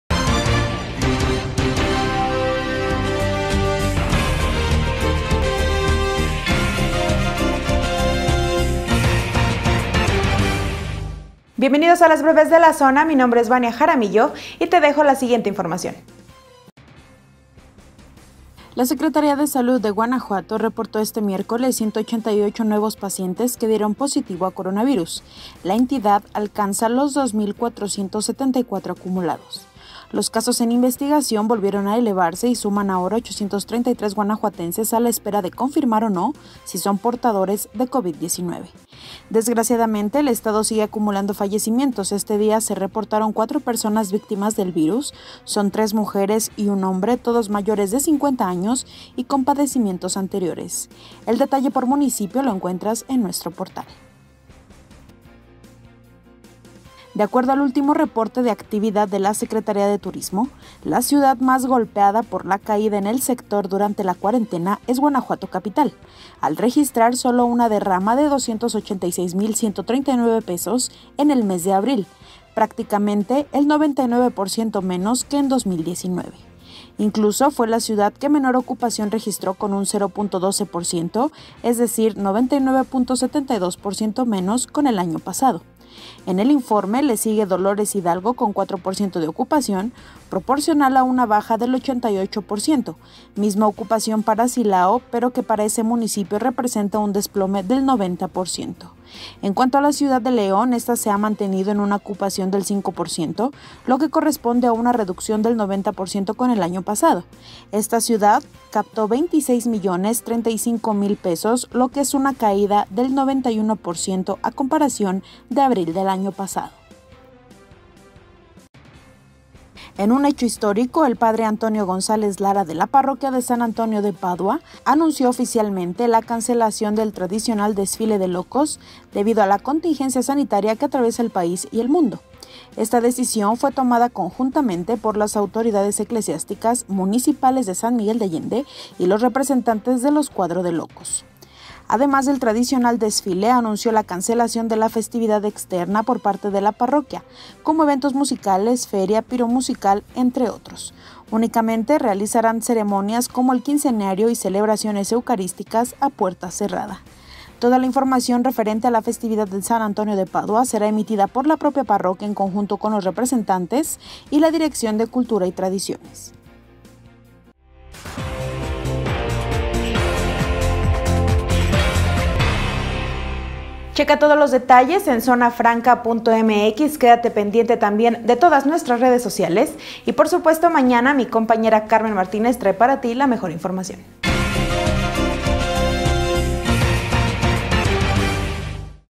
Se desploma turismo en Guanajuato capital | Breves de la Zona
BrevesDeLaZona📰 | Te dejamos las noticias más destacadas de esta noche
- Anuncian cancelación del Desfile de Locos, en San Miguel de Allende.
- En el día tres de reactivación, Guanajuato registra 188 nuevos pacientes de COVID19.
- Se desploma turismo en Guanajuato capital: recibió apenas 238 mil pesos en abril.